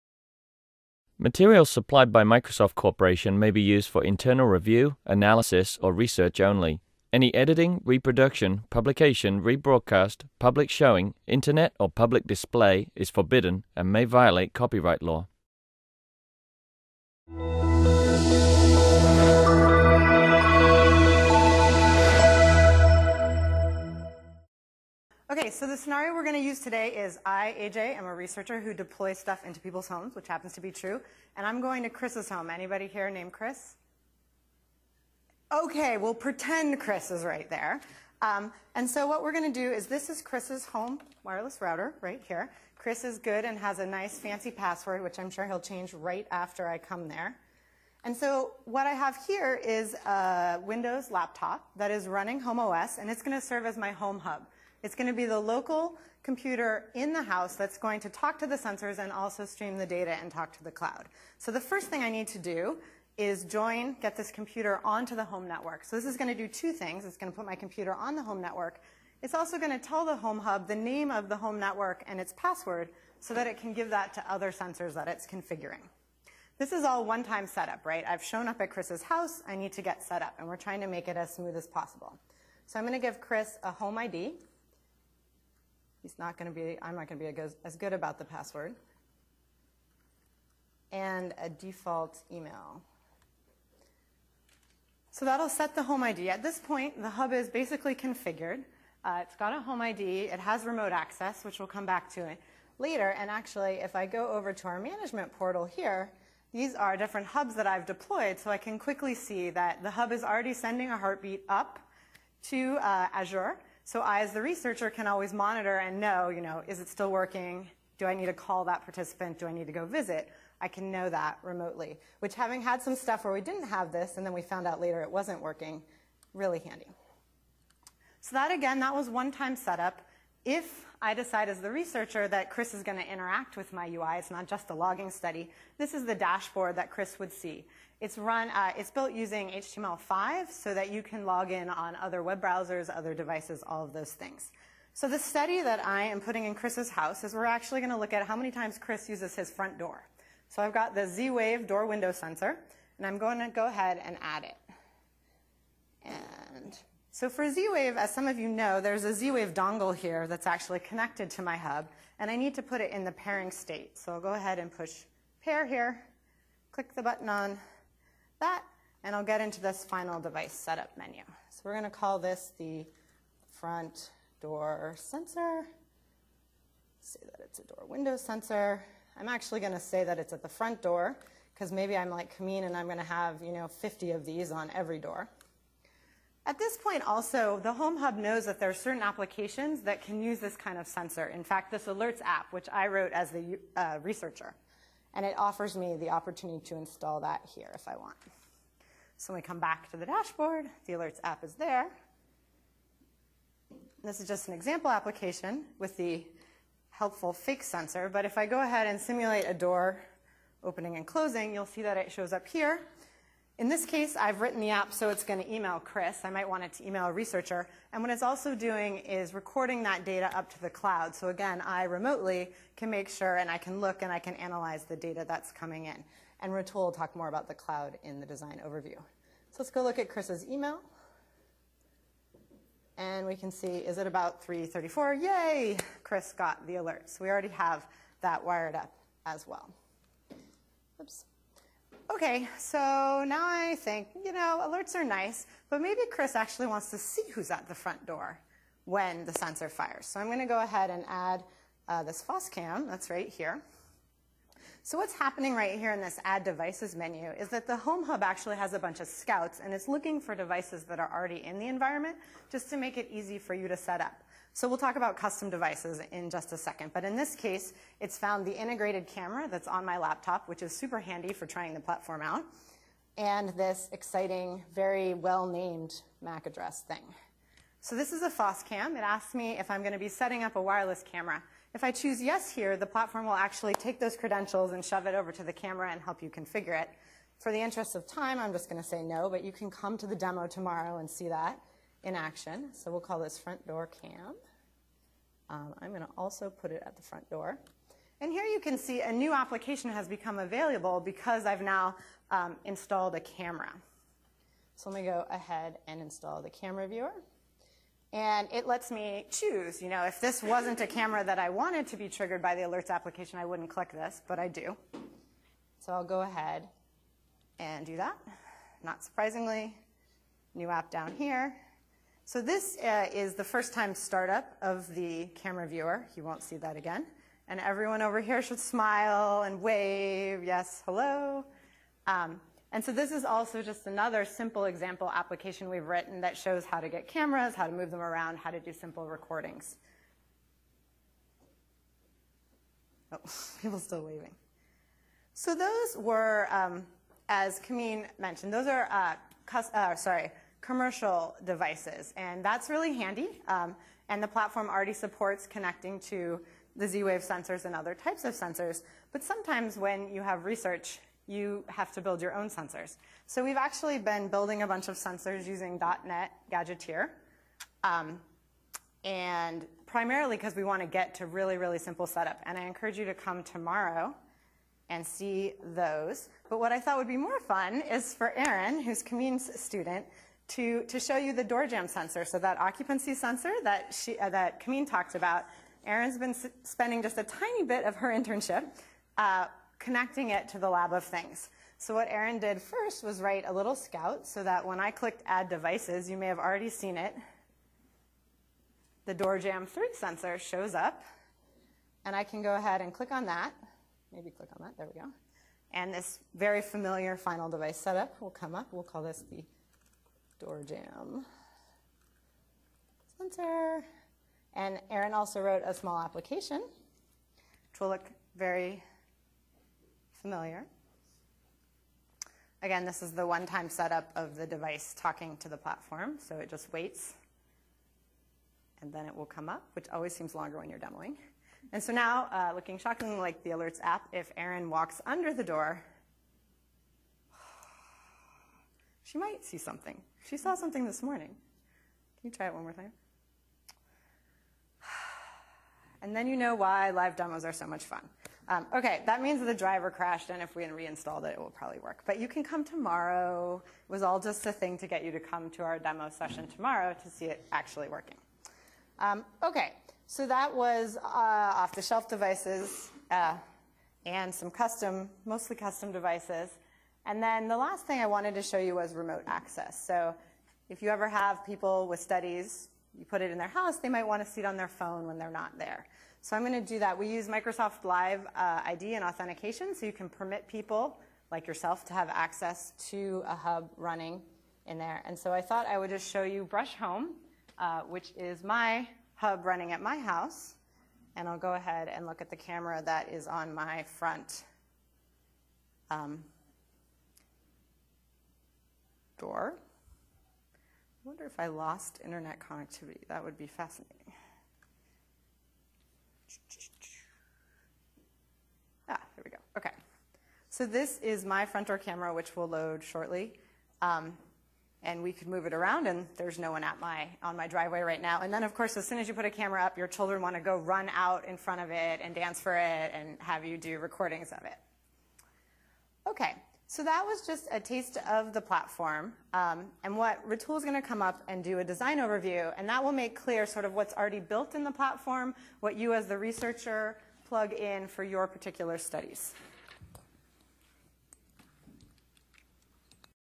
Towards a Research Platform for Internet of Things (IoT): A.J. Brush
We are living in a world of connected devices. These devices can take a number of shapes and forms: from tablet computers and smartphones, to sensors in the home, to embedded and wearable computers. Increasingly, there is interest in deploying devices and sensors in homes, buildings, vehicles, and any other spaces where human activity occurs. Most of these devices require software architectures and cloud services that are generally developed for either a specific device or class of devices. The prototyping and development of new devices requires new architectures and services, which consume precious research resources. To alleviate this barrier in research, a flexible platform with expandable device interface as well as programmatic access to device data and functionality, are required. In this session, researchers from academia and Microsoft Research will talk about research challenges in connected devices in the home and beyond, along with an overview of the upcoming HomeOS platform.